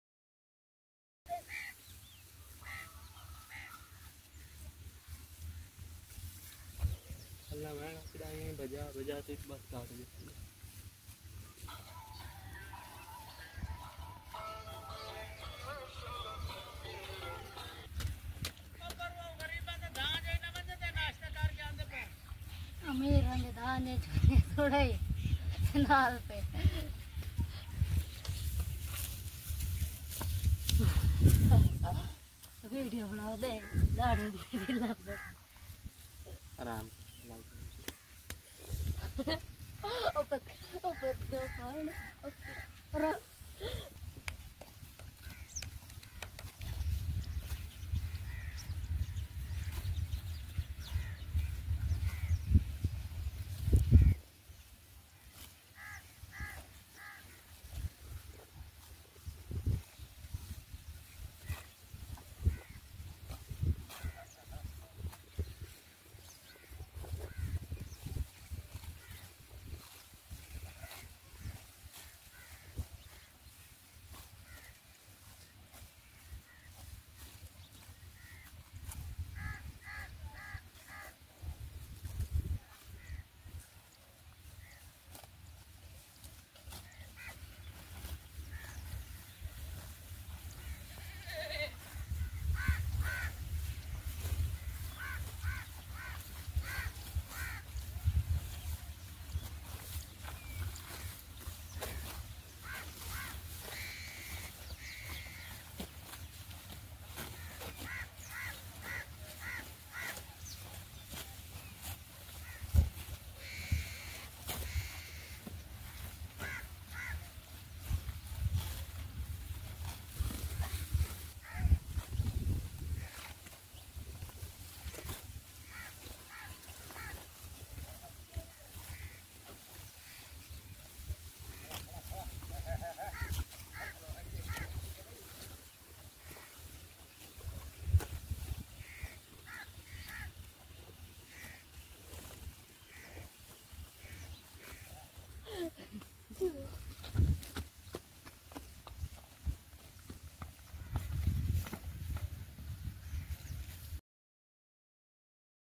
Lush Green Pasture And Herd of Cow and Buffalos
Assalam-O-Alaikum EveryOne !

A seen of happy herd those in lush green Pasture